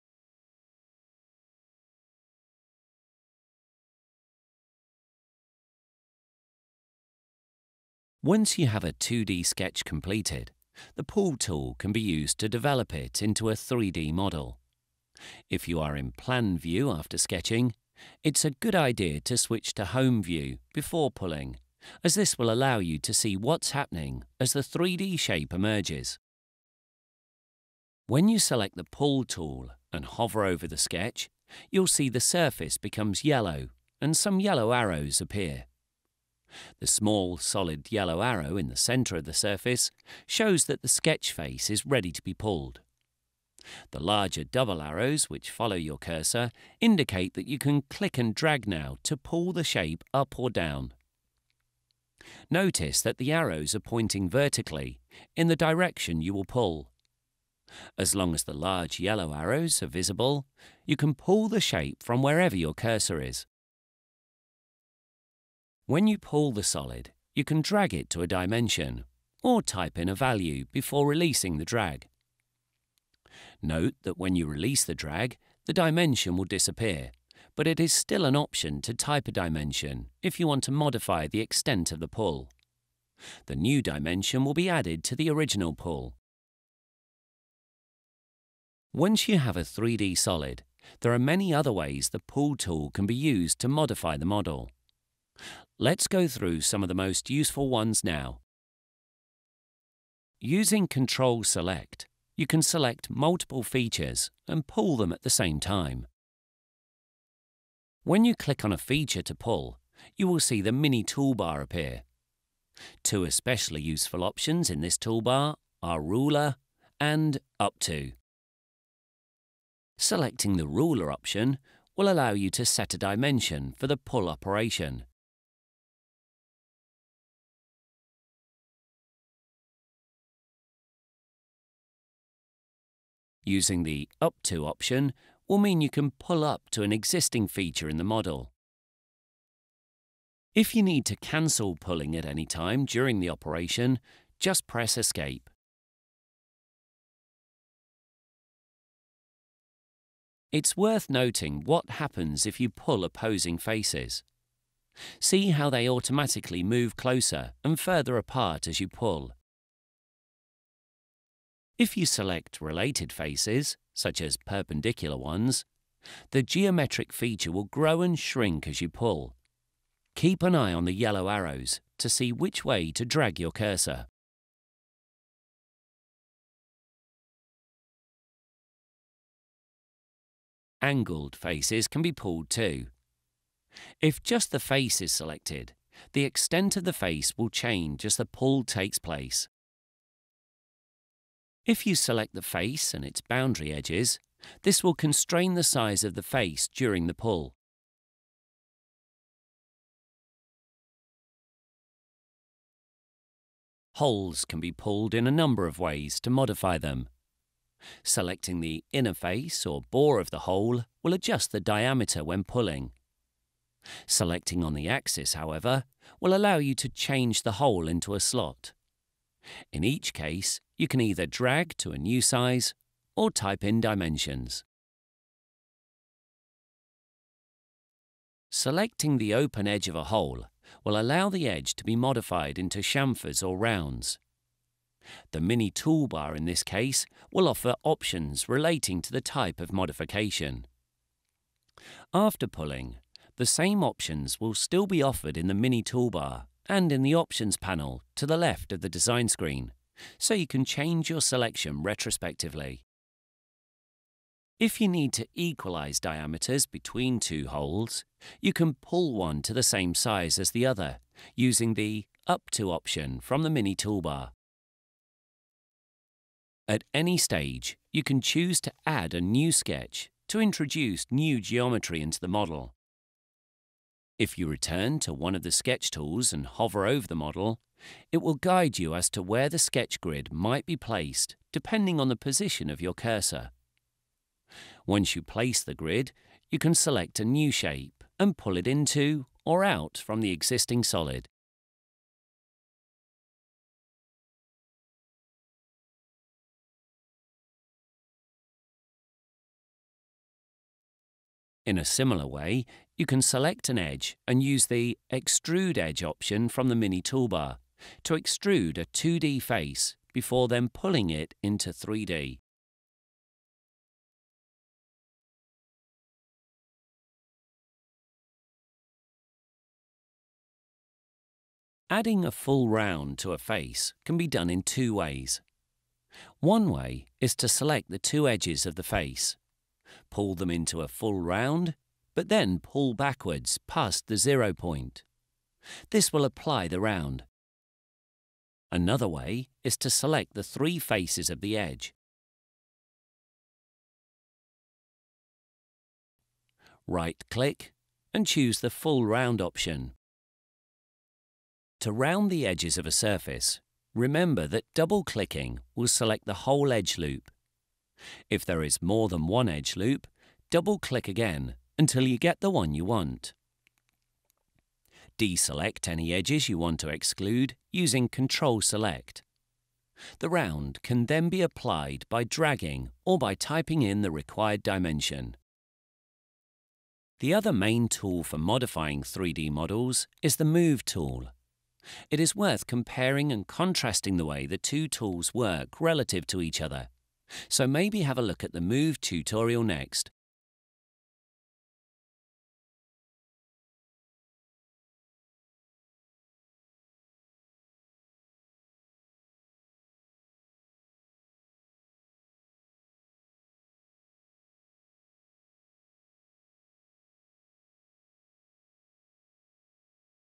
DesignSpark Mechanical - Basics - 4. The Pull Tool - Full tutorial
If you’re a first-time user, our video tutorials will teach you all you need to start designing with DesignSpark Mechanical – This video features guidance on how to use the pull tool.